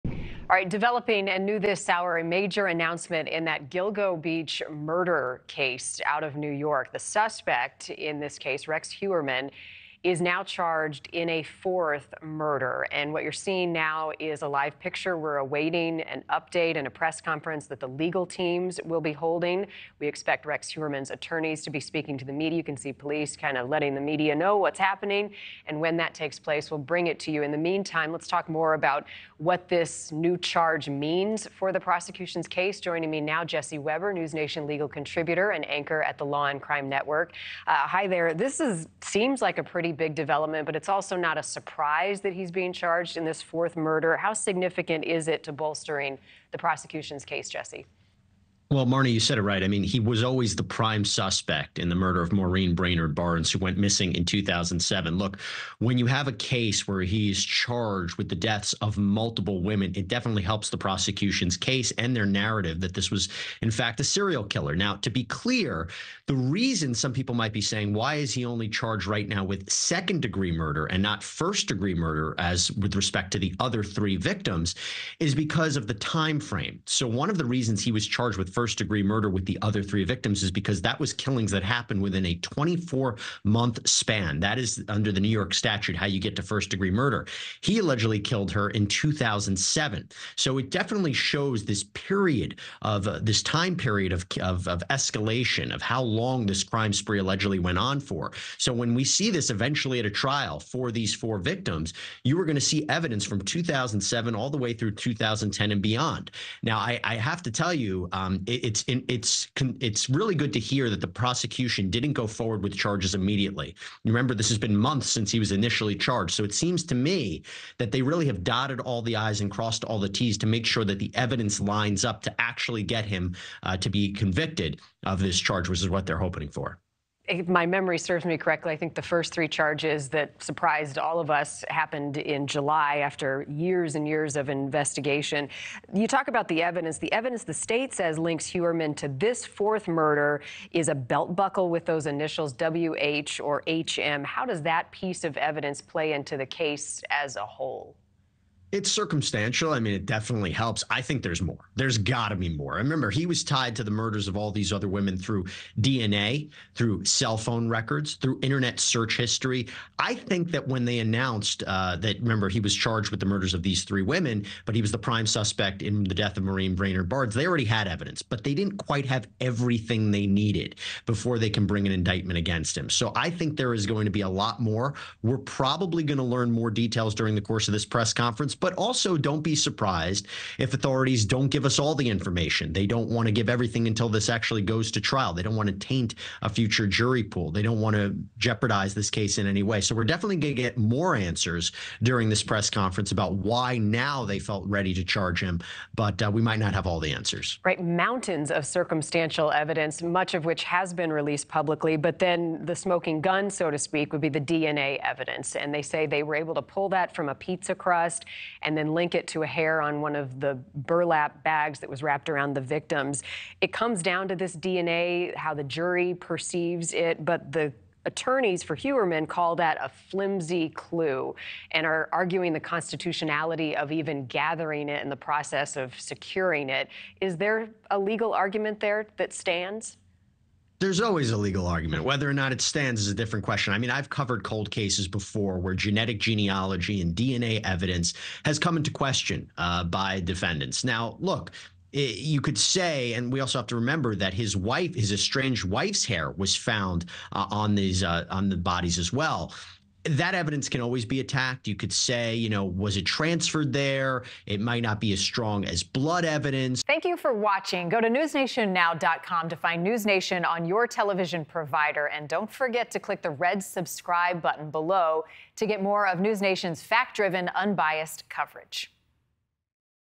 Rex Heuermann was always primary suspect in fourth murder: Journalist | NewsNation Live
Gilgo Beach serial killing suspect Rex Heuermann has been charged with killing 25-year-old Maureen Brainard-Barnes, the final victim of the Gilgo four. Jesse Weber discusses why Heuermann is being charged with 2nd-degree murder instead of 1st-degree murder.